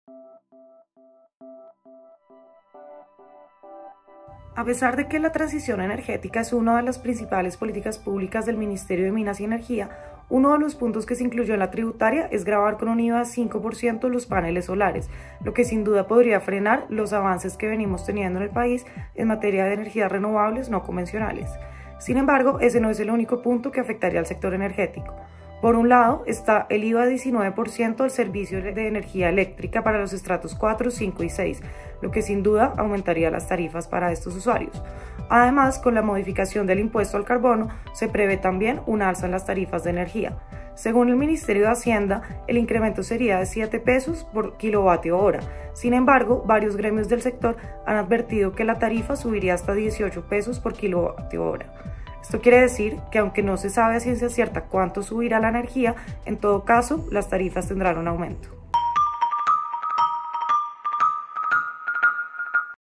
A pesar de la transición energética, tributaria gravaría energía solar
A pesar de la política de transición energética, se gravan los paneles solares. Además, aumentarían las tarifas de energía por IVA en el servicio público y el impuesto al carbono